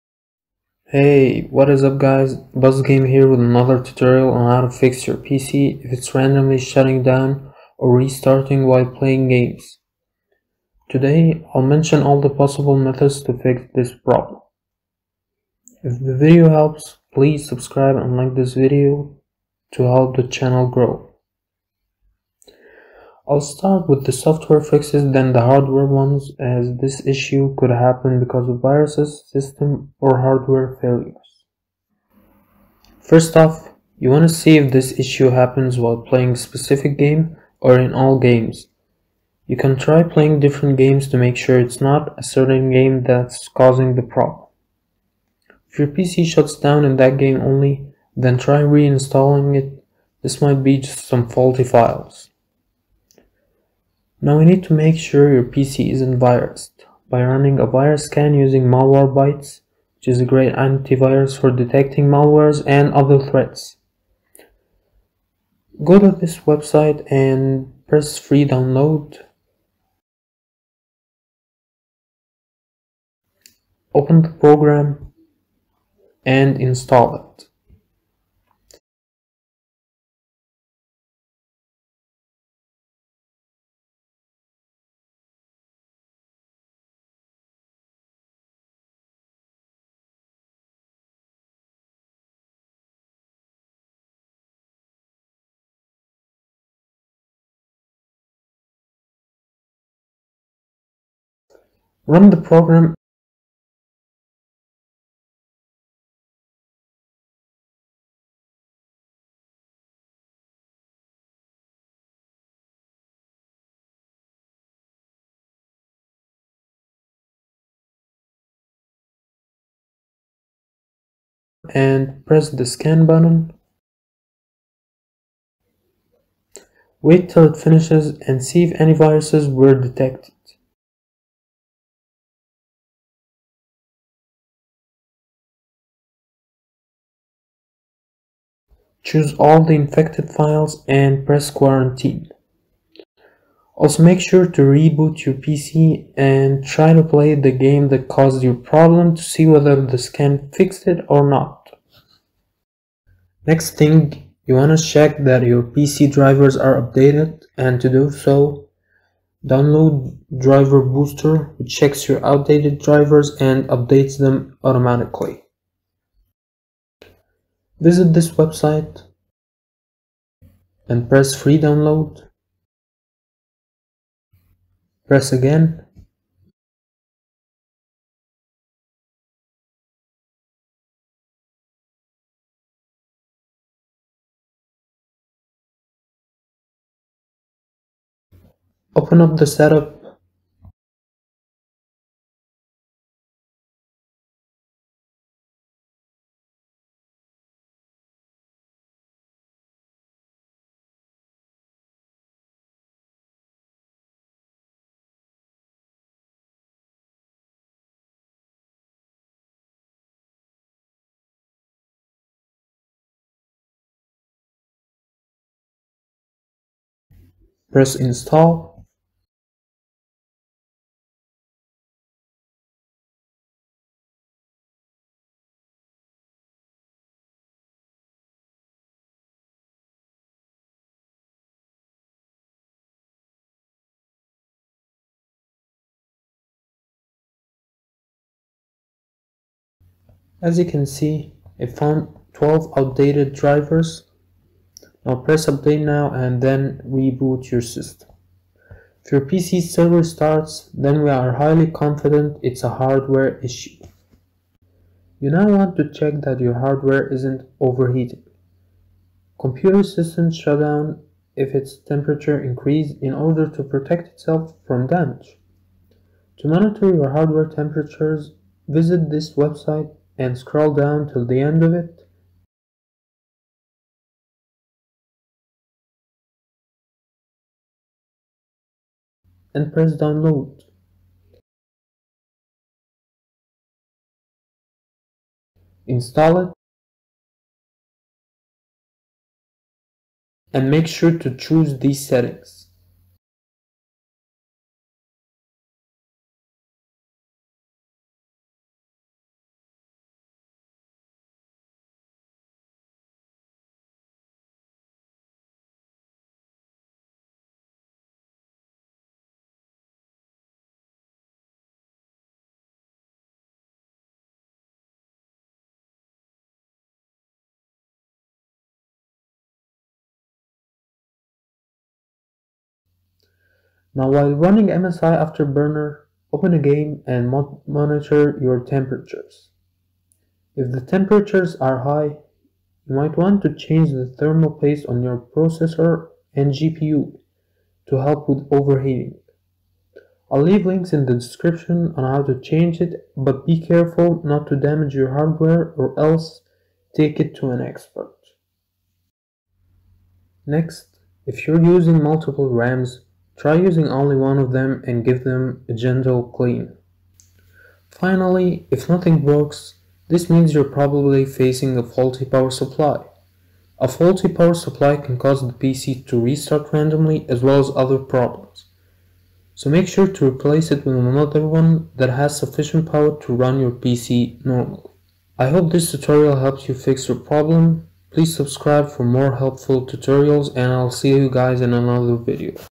How To Fix PC Restarting Randomly While Gaming (EASY GUIDE)
I show you all the possible ways to fix your PC if restarts randomly when playing games (Both SOFTWARE and HARDWARE fixes)

I personally suffered from the same exact problem where my PC would just restart when playing games but after a lot of troubleshooting I narrowed it down and it turned out to be that my power supply was going bad . So this video is a demonstration of the troubleshooting
I did to find the problem and fix it .

Additional fixes:
-Try searching for "Edit power plan" in your windows search bar and press "Change advanced power settings" and scroll down till you see "Processor power management" then change both Minimum and Maximum processor state to 95%.

-I recommend before replacing your power supply or motherboard to try another hard drive and install a clean Windows 10 version and try playing a game.
A faulty power supply can cause your PC to shut down on its own, also the motherboard may cause random restarts if it's damaged but that's very unlikely .